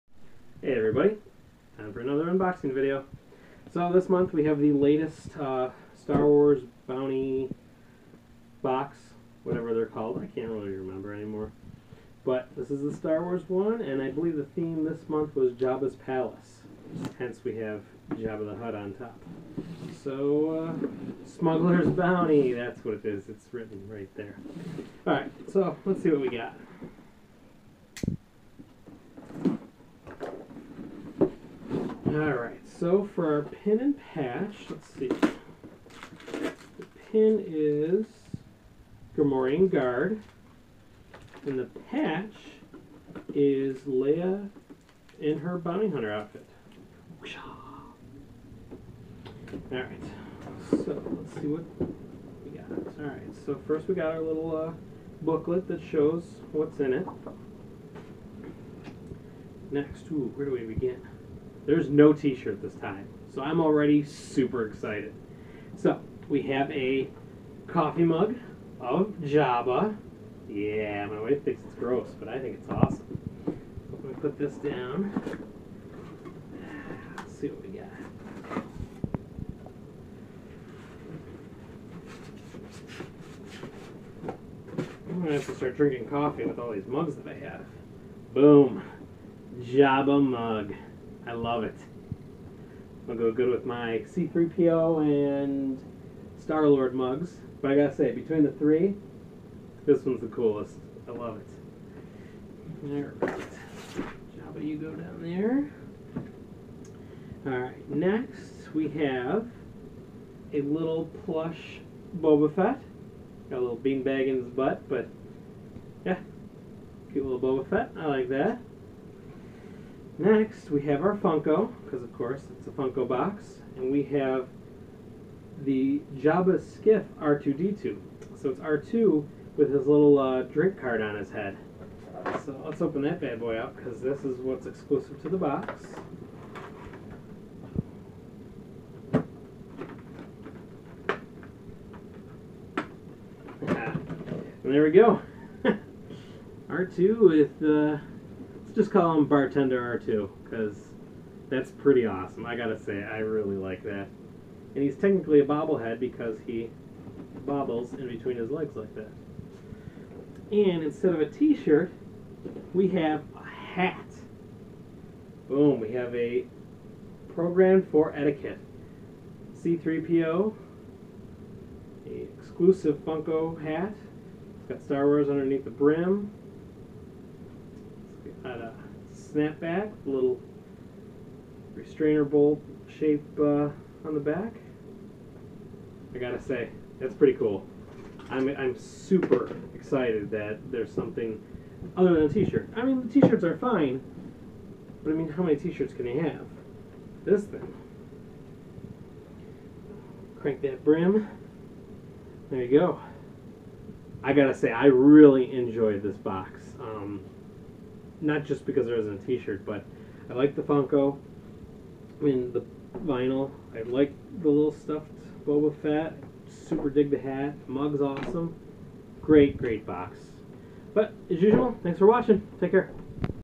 Smuggler's Bounty Jabba's Palace Unboxing
Unboxing the Funko Star Wars Smuggler's Bounty for July 2016. The Jabba's Palace box